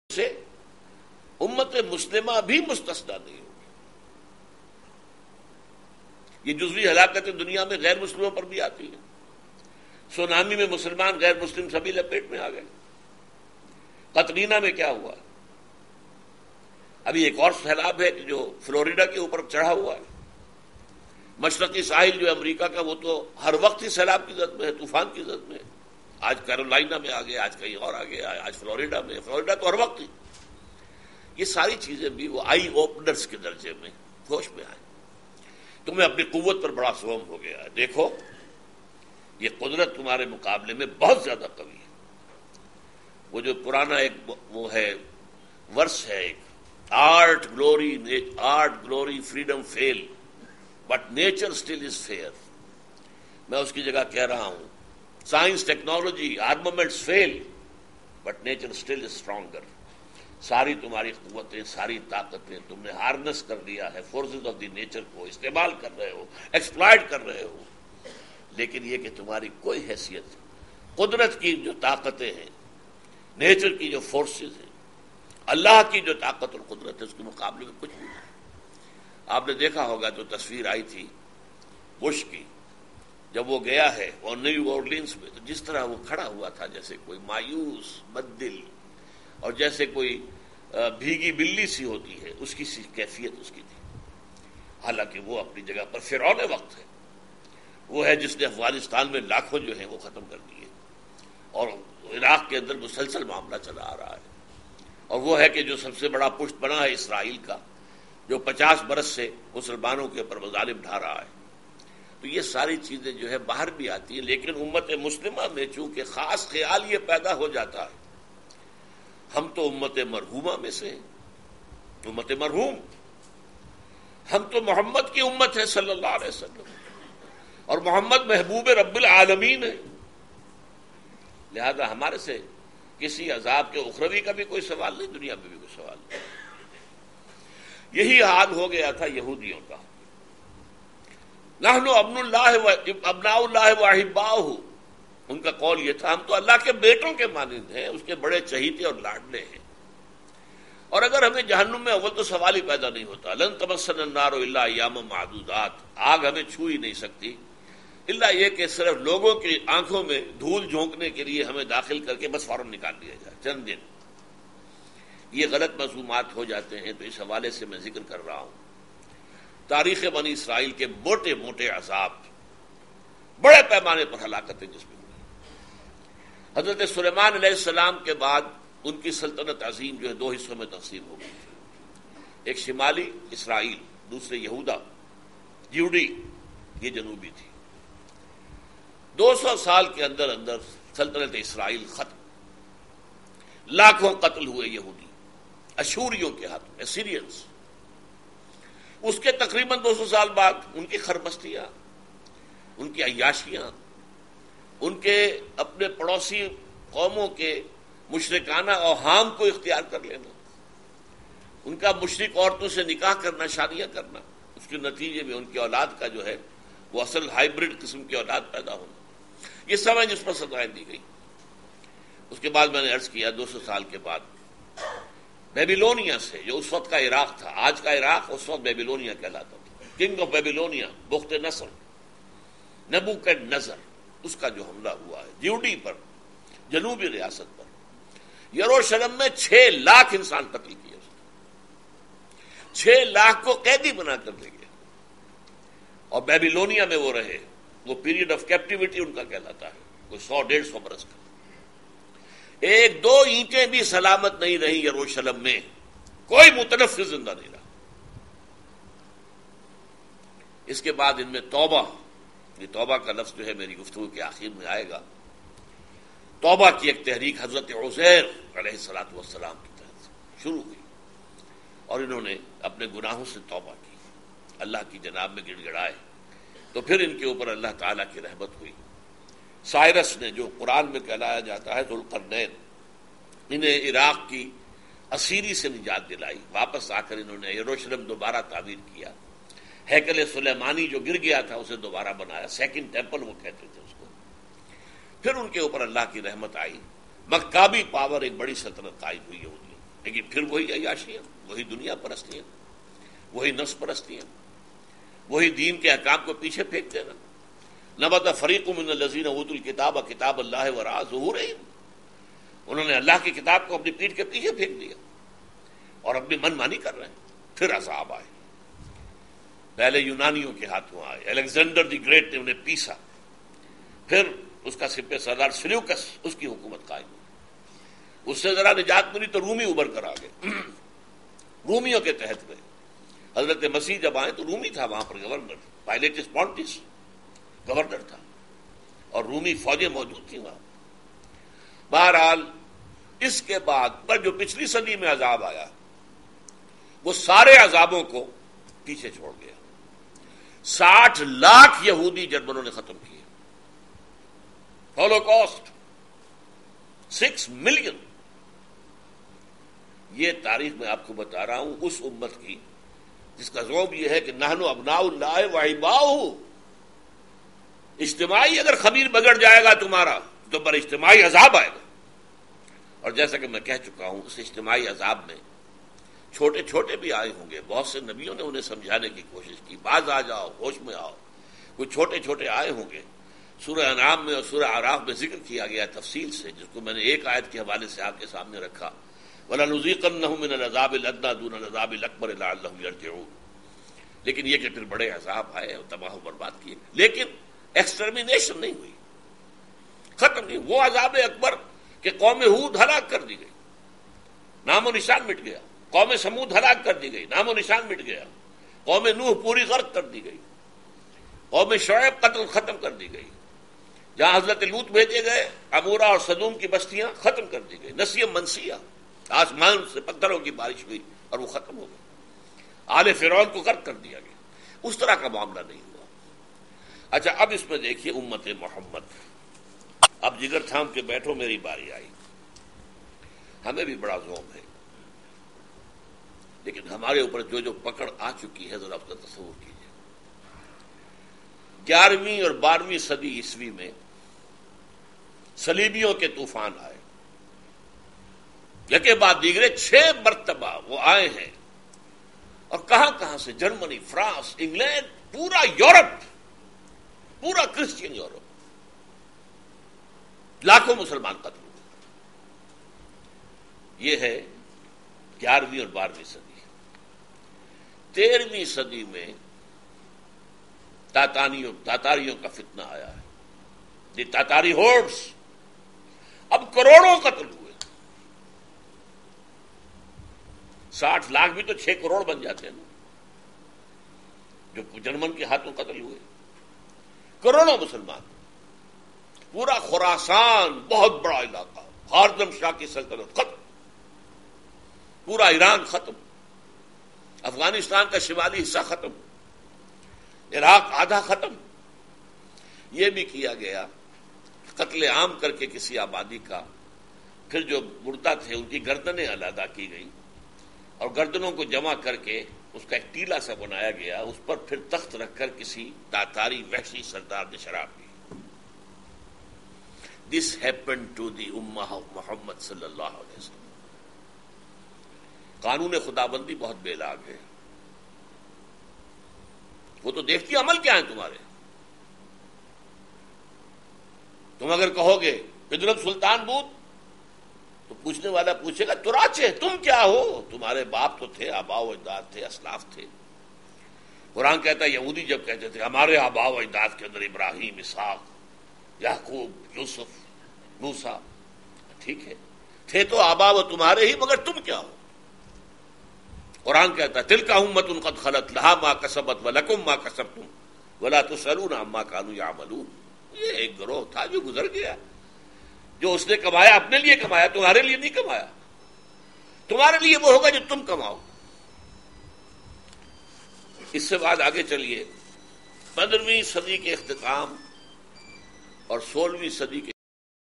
(5/8) Pakistan Mein Zalzala, Aafet Ya Azab
A public lecture by Dr. Israr Ahmed

Tanzeem-e-Islami Quran Academy Markazi Anjuman e Khuddam ul Qur'an Lahore IRF Peace TV Urdu Qtv Dr. Zakir Naik